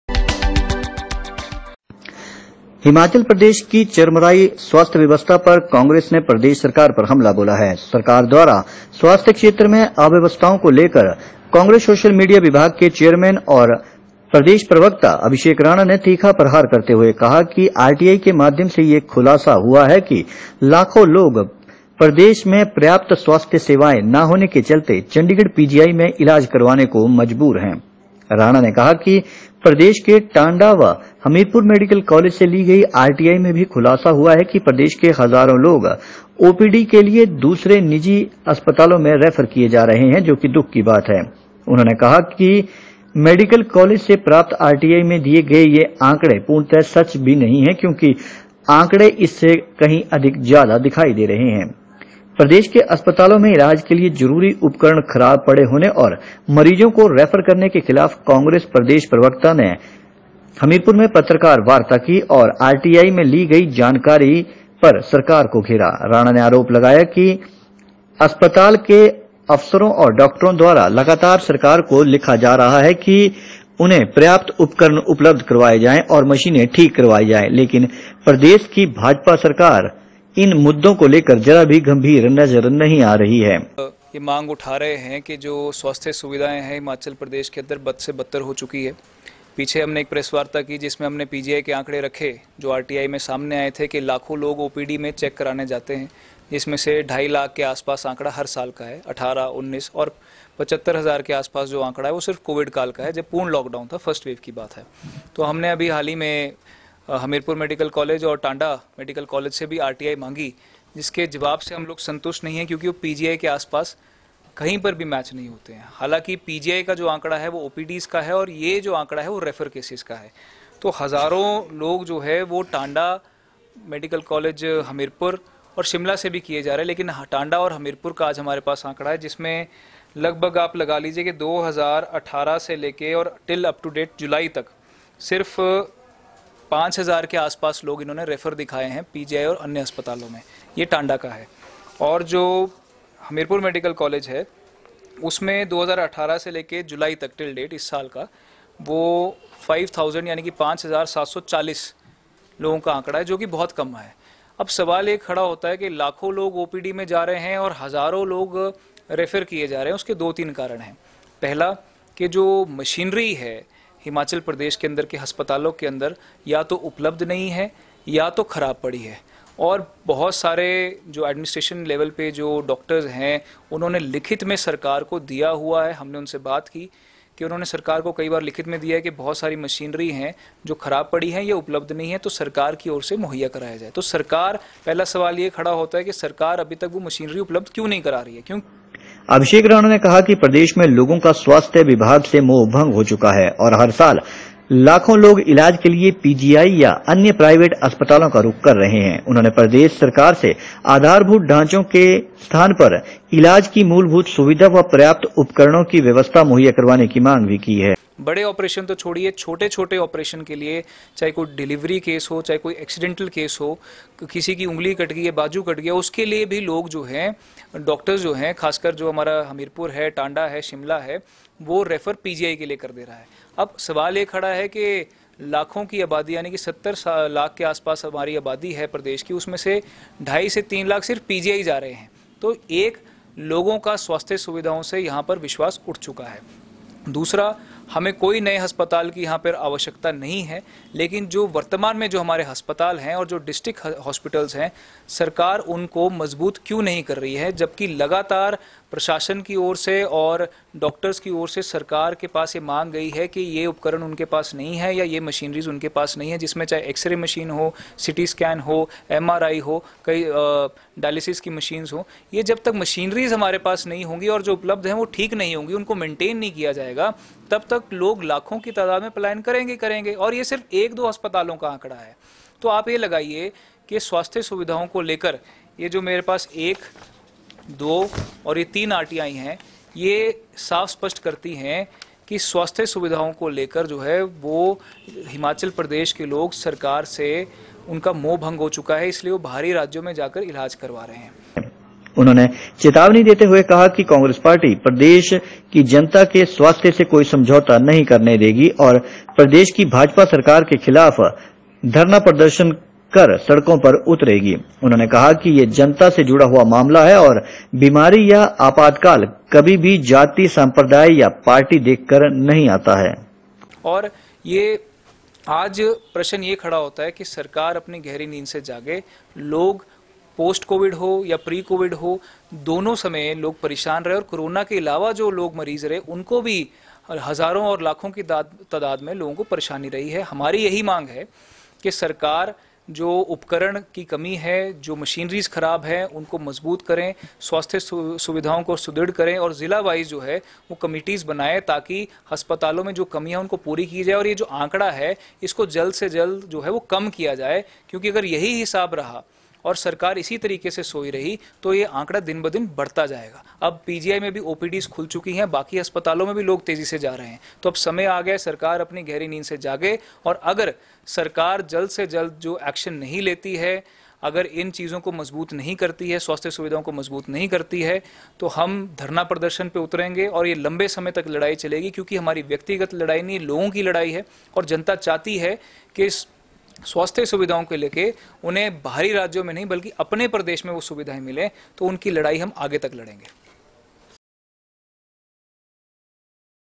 Hamirpurहिमाचल के लाखों लोग इलाज के लिए बाहरी राज्यों मेंजाने को विवशः काग्रेंस प्रवक्ता अभिषेक राणा
हिमाचल के लाखों लोग इलाज के लिए बाहरी राज्यों में जाने को विवशः काग्रेंस प्रवक्ता अभिषेक राणा
पीजीआई से ली गई आरटीआई में खुलाया हर साल लाखों हिमाचली लोग पहुंच रहे इलाज को
काग्रेंस प्रवक्ता ने कहा- लाखों लोग ओपीडी और हजारों लोग इमरजेंसी के लिए किए जा रहे दूसरे अस्पतालों में रेफर @ Rajinder rana @IndianNationalCongress @ Abhishek Rana